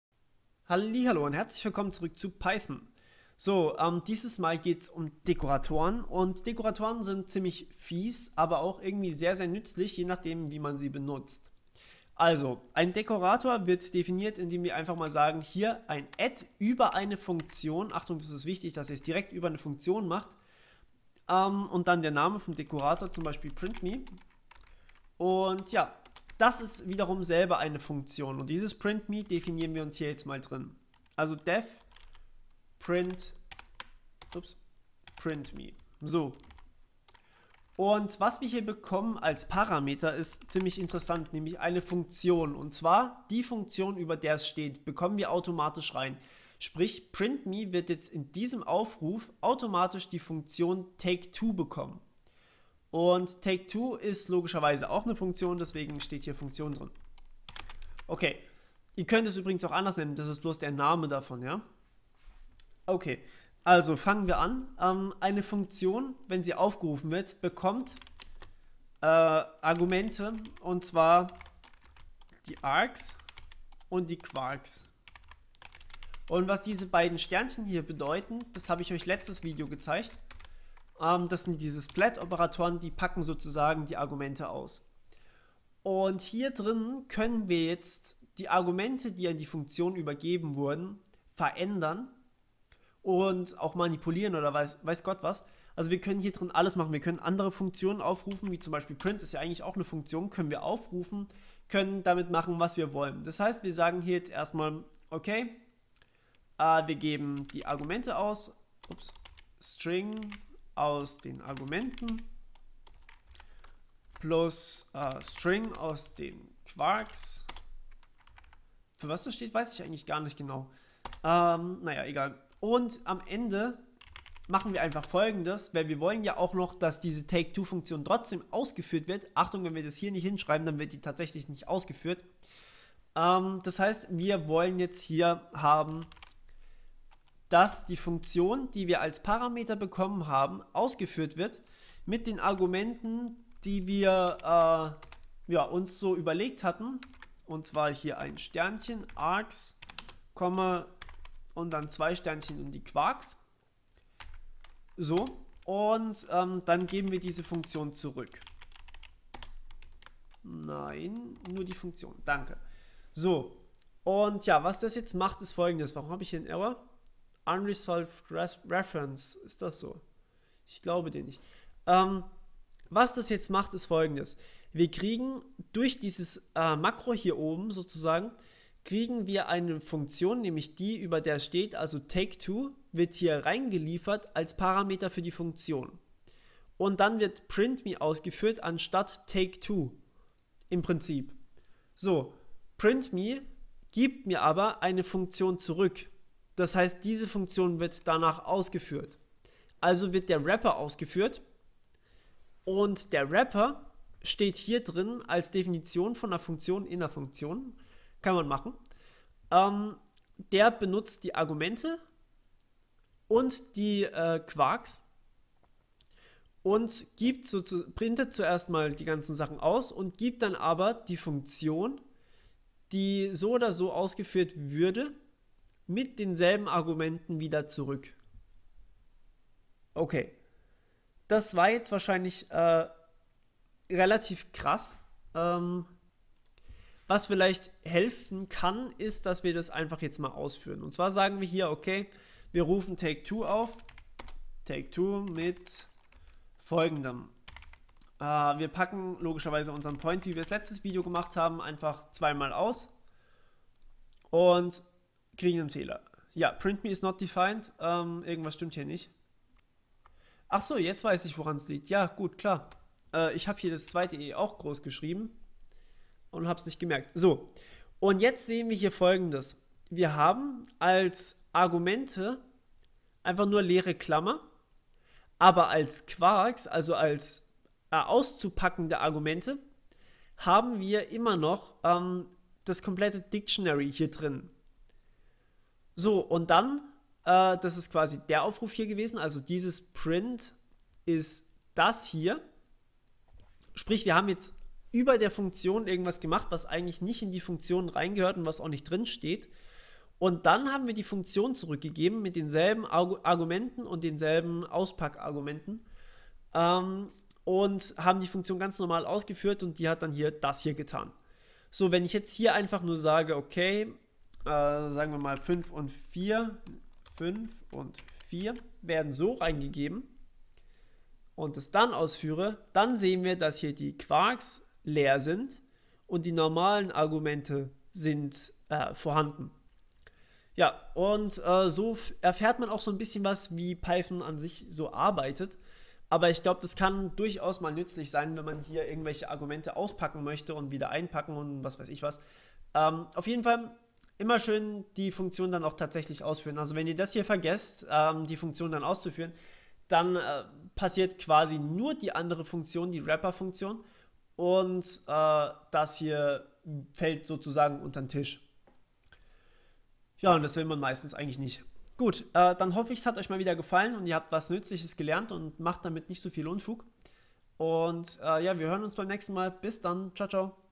Python Tutorial #45 - Dekoratoren
In diesem Tutorial zeige ich euch, was Dekoratoren sind.